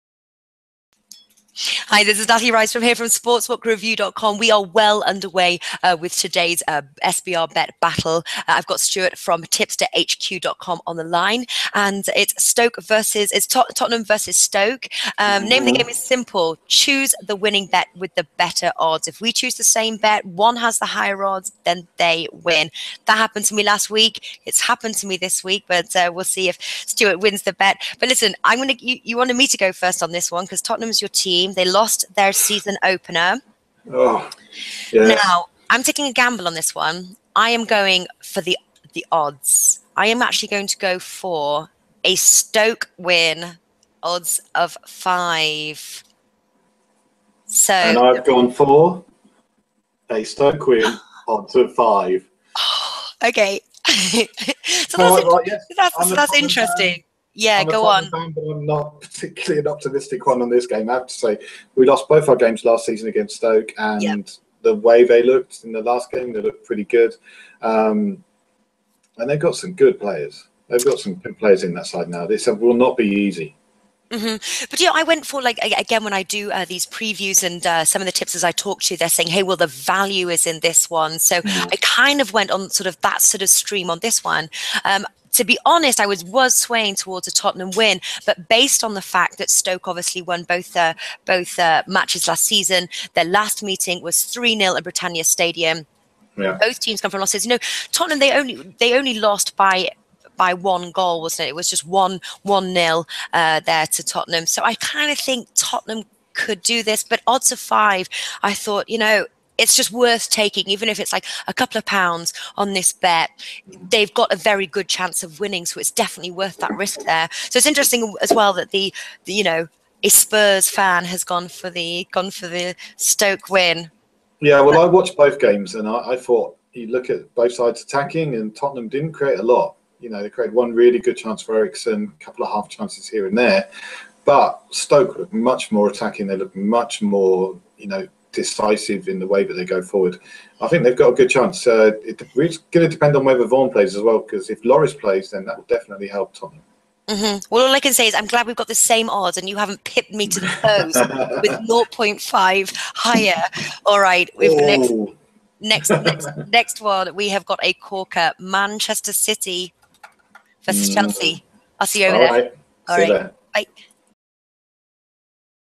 Tottenham vs Stoke 15.08.15 | Premier League | SBR Bet Battle
In this sports video our guests  preview the English Premier League EPL football match Tottenham vs Stoke playing on Saturday 15th August 2015

Sportsbook Review's Natalie Rydstrom and Stuart from TalkingTipsters.com and TipsterHQ.com come together to battle it out with their best bet. The name of the game is simple, each person will present their tip and explain why they chose it, then after the match is played the tip has to both win and have the higher odds for the person to win that round. SBR Bet Battle will take place each week covering all Premier League matches for the 2015-16 season.